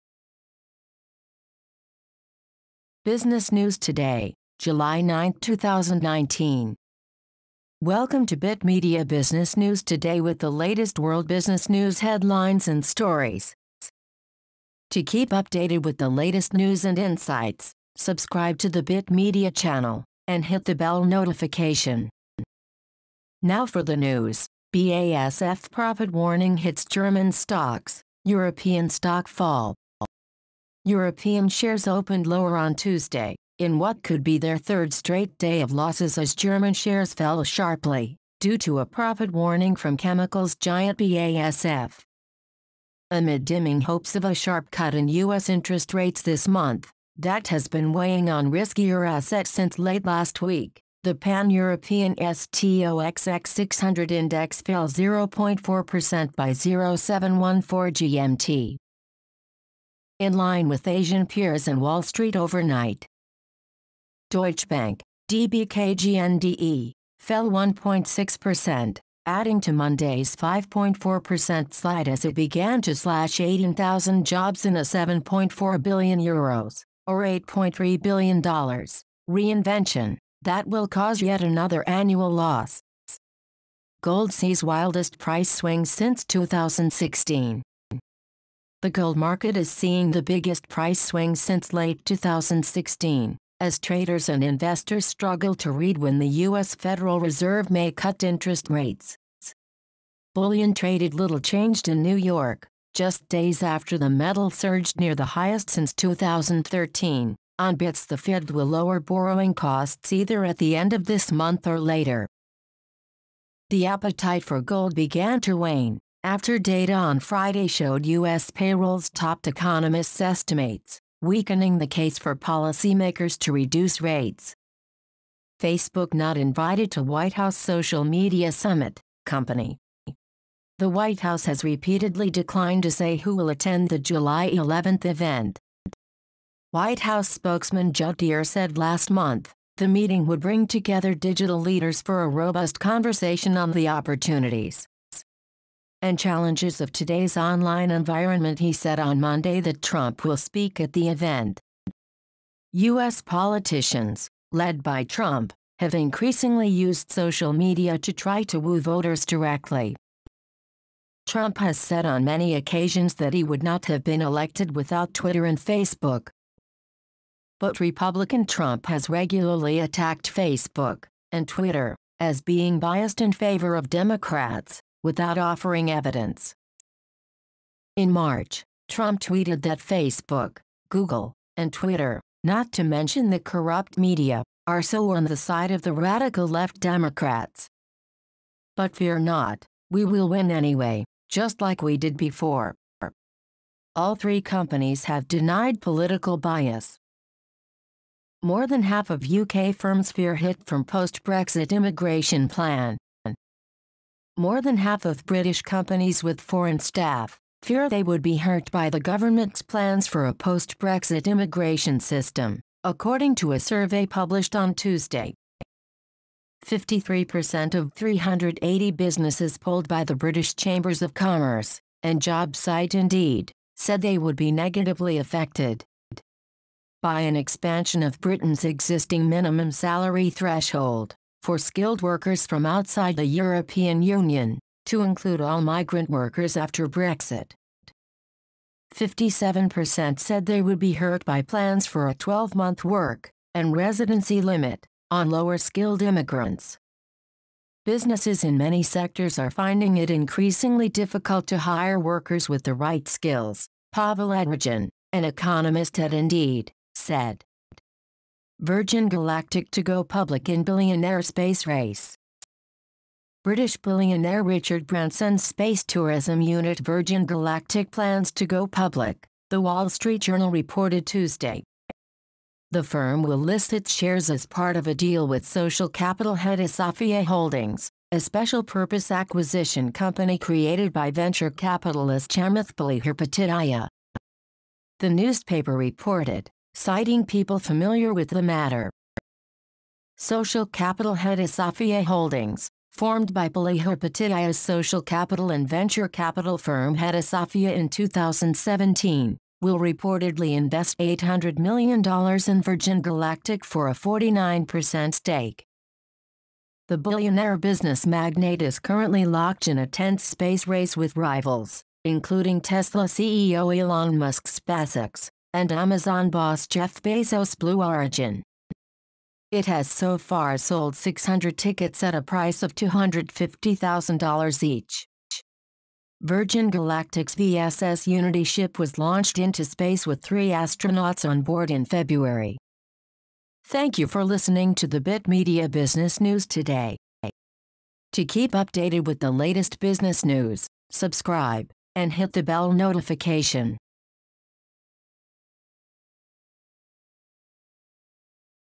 Business News Today - 9 July 2019 - Gold sees wildest price swings since 2016
Welcome to BitMedia Business News Today with the latest World Business News headlines and stories.

Now for the news. BASF profit warning hits German stocks, European stock fall

European shares opened lower on Tuesday, in what could be their third straight day of losses as German shares fell sharply, due to a profit warning from chemicals giant BASF.

Amid dimming hopes of a sharp cut in U.S. interest rates this month, that has been weighing on riskier assets since late last week, the pan-European STOXX 600 index fell 0.4% by 0714 GMT, in line with Asian peers and Wall Street overnight.

Deutsche Bank (DBKGn.DE) fell 1.6%, adding to Monday’s 5.4% slide as it began to slash 18,000 jobs in a 7.4 billion euro, or $8.3 billion, “reinvention”, that will cause yet another annual loss.

Gold sees wildest price swings since 2016

The gold market is seeing the biggest price swings since late 2016, as traders and investors struggled to read when the US Federal Reserve may cut interest rates.

Bullion traded little changed in New York, just days after the metal surged near the highest since 2013, on bets the Fed will lower borrowing costs either at the end of this month or later. The appetite for gold began to wane, after data on Friday showed US payrolls topped economists’ estimates, weakening the case for policymakers to reduce rates.

Facebook not invited to White House social media summit, Company.

The White House has repeatedly declined to say who will attend the July 11 event.

White House spokesman Judd Deere said last month, the meeting would “bring together digital leaders for a robust conversation on the opportunities, and challenges of today’s online environment.” He said on Monday that Trump will speak at the event.

U.S. politicians, led by Trump, have increasingly used social media to try to woo voters directly. Trump has said on many occasions that he would not have been elected without Twitter and Facebook.

But Republican Trump has regularly attacked Facebook, and Twitter, as being “biased” in favor of Democrats, without offering evidence.

In March, Trump tweeted that “Facebook, Google, and Twitter, not to mention the Corrupt Media, are sooo on the side of the Radical Left Democrats. But fear not, we will win anyway, just like we did before!”

All three companies have denied political bias.

More than half of UK firms fear hit from post-Brexit immigration plan.

More than half of British companies with foreign staff, fear they would be hurt by the government's plans for a post-Brexit immigration system, according to a survey published on Tuesday.

Fifty-three percent of 380 businesses polled by the British Chambers of Commerce, and job site Indeed, said they would be negatively affected, by an expansion of Britain's existing minimum salary threshold, for skilled workers from outside the European Union, to include all migrant workers after Brexit.

Fifty-seven percent said they would be hurt by plans for a 12-month work, and residency limit, on lower-skilled immigrants.

"Businesses in many sectors are finding it increasingly difficult to hire workers with the right skills," Pawel Adrjan, an economist at Indeed, said.

Virgin Galactic to go Public in Billionaire Space Race

British billionaire Richard Branson’s space tourism unit Virgin Galactic plans to go public, the Wall Street Journal reported Tuesday.

The firm will list its shares as part of a deal with Social Capital Hedosophia Holdings, a special-purpose acquisition company created by venture capitalist Chamath Palihapitiya, the newspaper reported, citing people familiar with the matter.

Social Capital Hedosophia Holdings, formed by Palihapitiya’s Social Capital and venture capital firm Hedosophia in 2017, will reportedly invest $800 million in Virgin Galactic for a 49% stake.

The billionaire business magnate is currently locked in a tense space race with rivals, including Tesla CEO Elon Musk’s SpaceX, and Amazon boss Jeff Bezos’ Blue Origin.

It has so far sold 600 tickets at a price of $250,000 each. Virgin Galactic’s V.S.S Unity ship was launched into space with three astronauts on board in February.

Thank you for listening to the BitMedia Business news today.